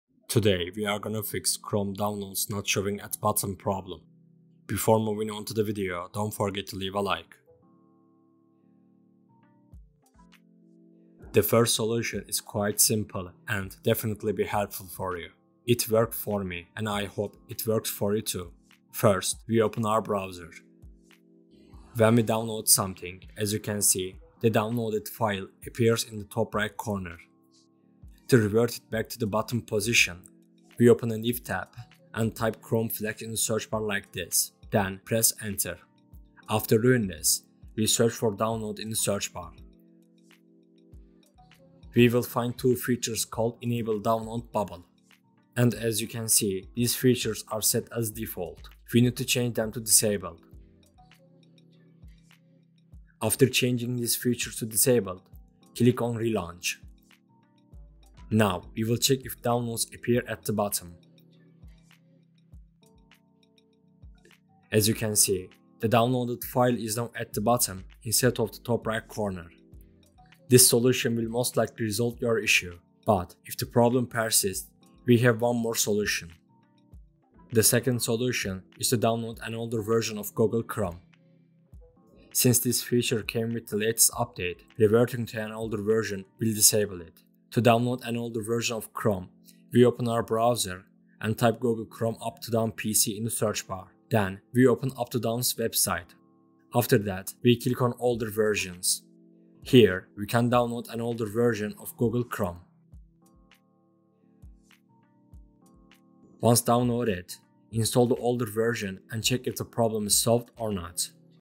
GOOGLE CHROME DOWNLOADS NOT SHOWING AT BOTTOM [FIXED]
Here's how to fix google chrome downloads not showing at bottom. This error is searched as downloads not showing at bottom of chrome and spotify not showing play bar. And in this video I showed you how you can fix this download bar not showing in chrome in 2 minutes.

Video Parts:
00:00 Info&Intro
00:09 Disable Download Bubble
01:16 Download an older version of Google Chrome